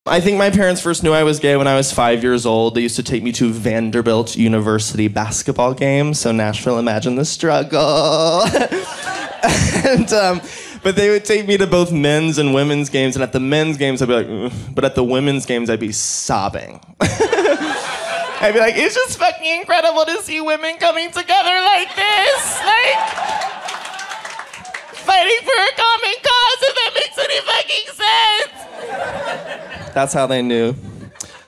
"At the women's games I'd be sobbing." 🎤: John Early
How women’s basketball helped John Early come out.